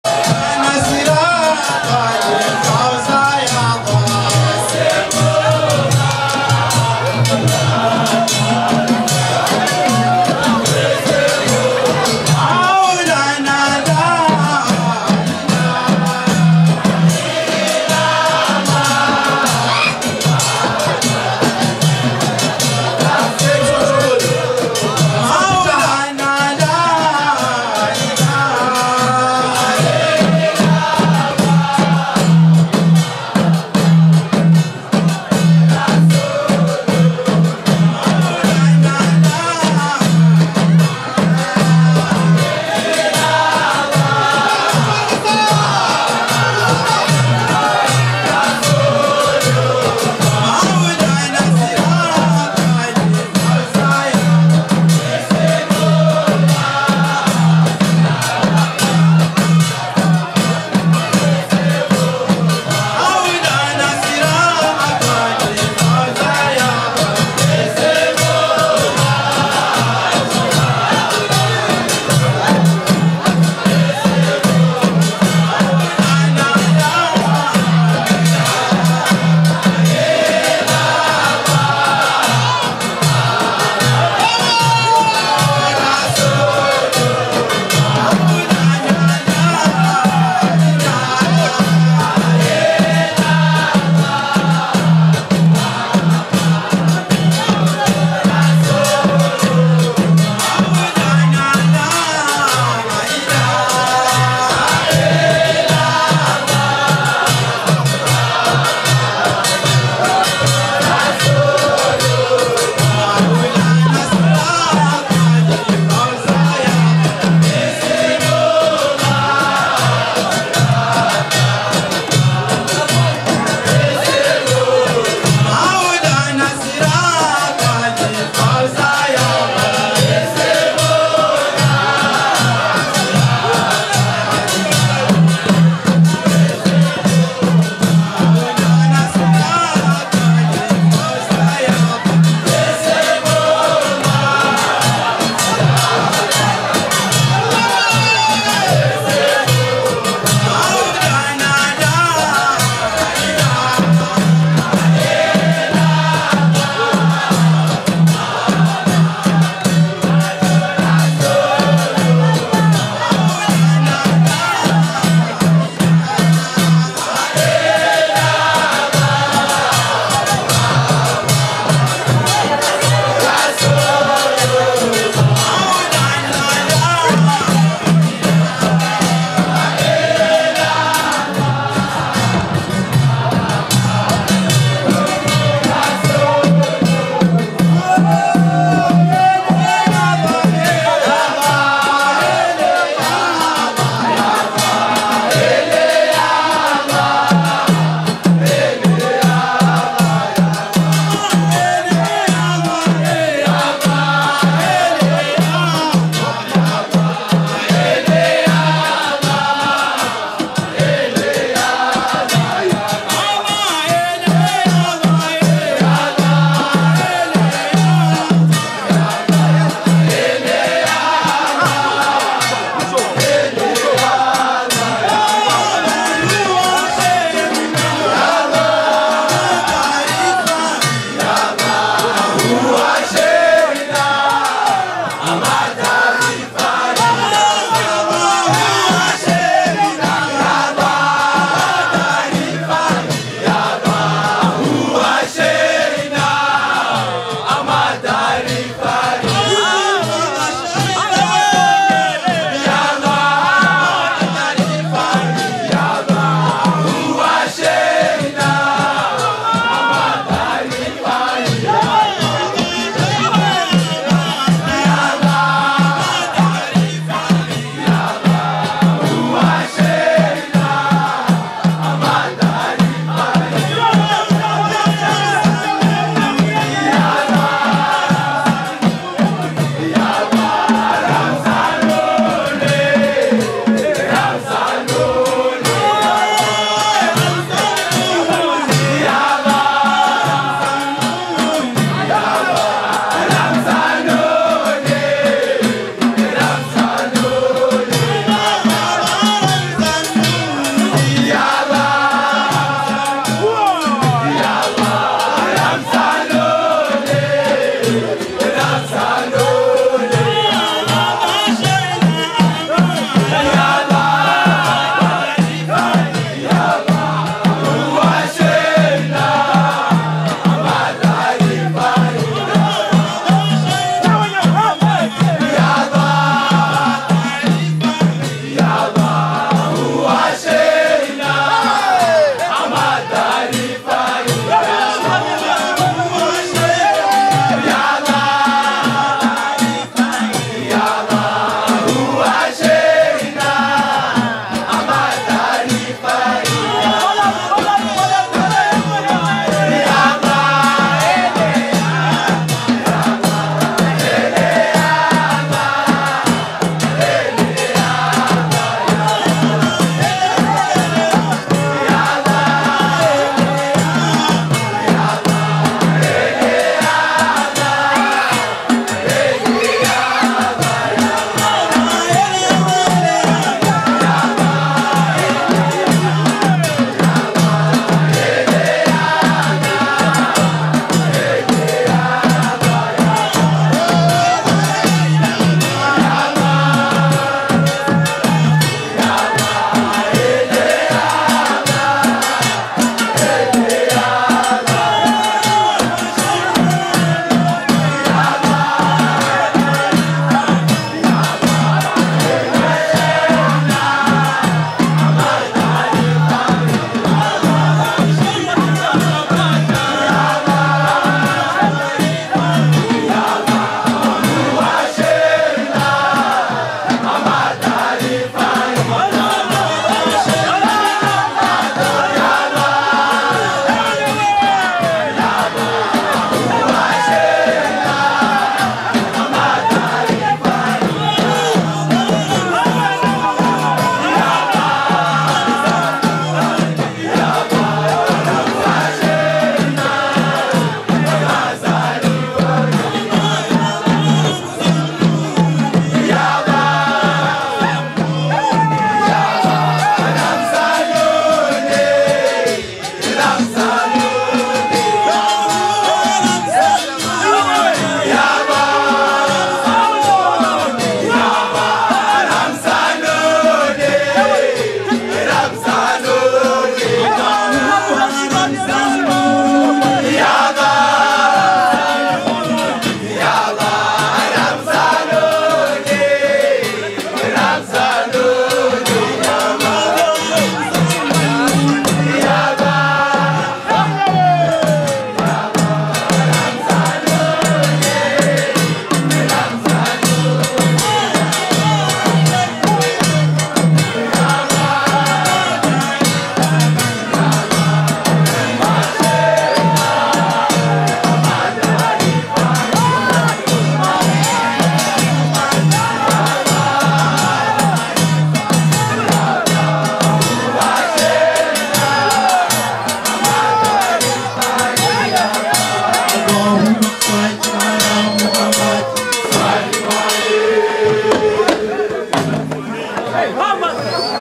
Moulidi fete de l'Aid 2023
30 avril 2023
fête de l'aïd après 1 mois de Ramadan